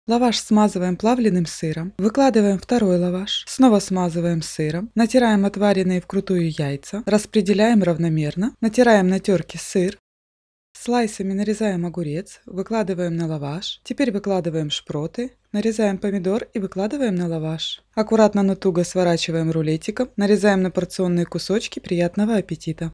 😲Вы будете в ШОКЕ! БЕРУ ЛАВАШ И ЗАВОРАЧИВАЮ В РУЛЕТ! ШПРОТЫ И Лаваш. Шпроты в лаваше, shorts
БЕРУ ЛАВАШ И ЗАВОРАЧИВАЮ В РУЛЕТ! ШПРОТЫ И Лаваш. Шпроты в лаваше. Sprats in Pita. shorts

Всем привет, друзья! Предлагаю Вашему вниманию рецепт приготовления вкусной закуски к любому праздничному столу. Рулет из лаваша с плавленым сыром обладает нежным вкусом, а благодаря огурцу и помидорам приобретает сочность и свежесть. Рулет из лаваша со шпротами, яйцом и сыром получается сытным и ароматным. Вкусная закуска, которую можно приготовить на скорую руку и подать как в будний день, так и к приходу неожиданных гостей. Отличная холодная закуска в новогоднем или праздничном меню.

Ингредиенты
Лаваш – 2шт.
Яйца – 2 шт.
Шпроты – 1 баночка (190 гр.)
Огурец – 1 шт.
Плавленный сыр янтарь – 160 гр.
Сыр твердый – 50 гр.

💖Желаю Вам приятного аппетита, хорошего настроения и всего самого наилучшего!